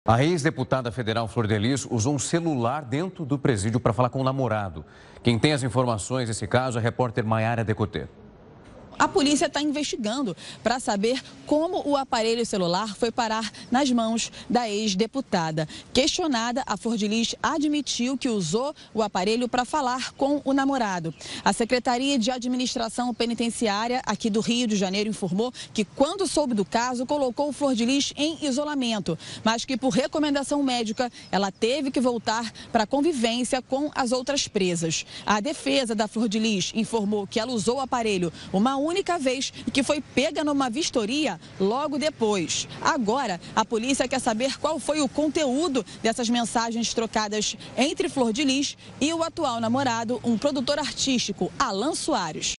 Celular é encontrado dentro da cela de Flordelis
A ex-deputada federal Flordelis usou um celular dentro do presídio para falar com o namorado.